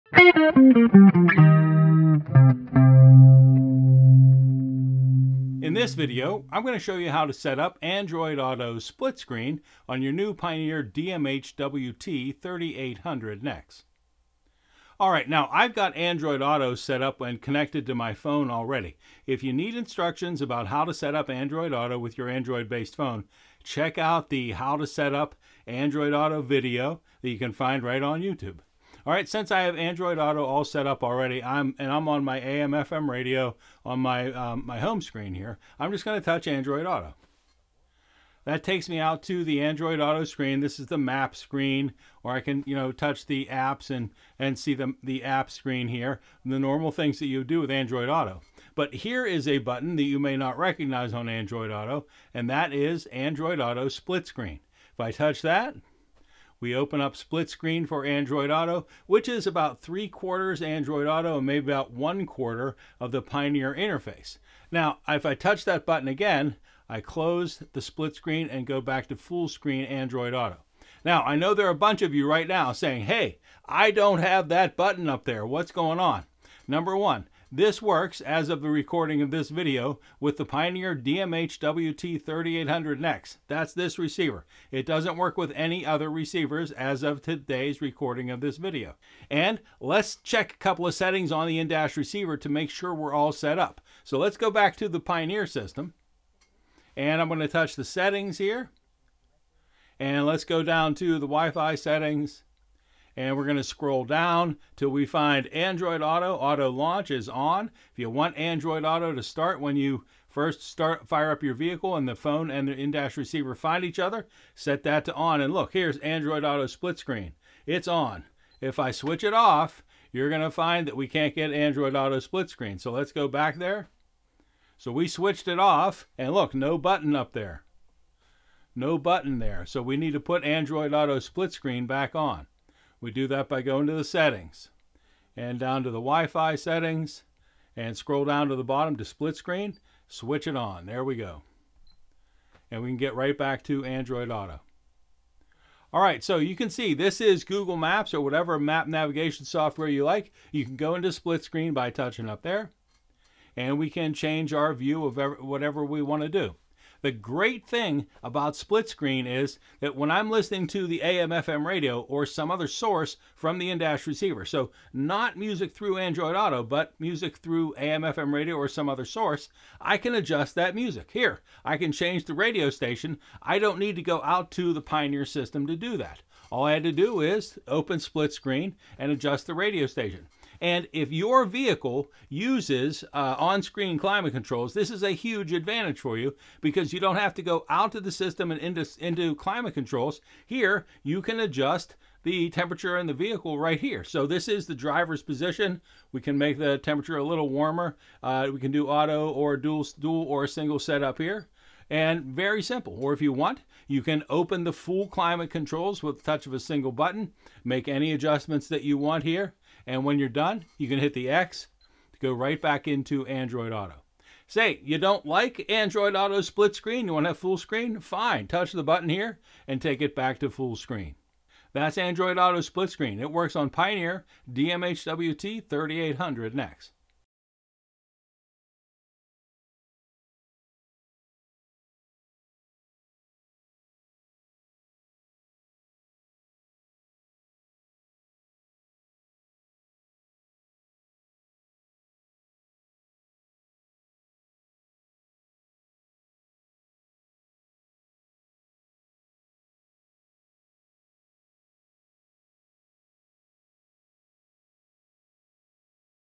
How to - Android Auto Split Screen - Pioneer DMH WT3800NEX
In this How To Video, you'll learn about the Pioneer DMH-WT3800NEX and How To set up Android Auto Split Screen from your compatible Android based phone.

DMH-WT3800NEX